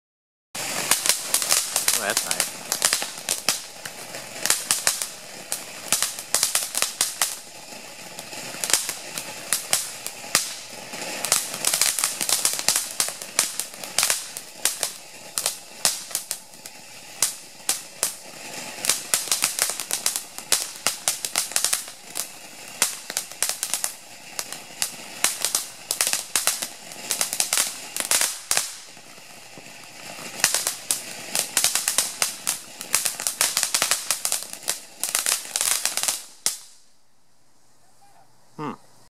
Firework Fountain
In my neighbor's yard in Toms River, New Jersey 7-4-18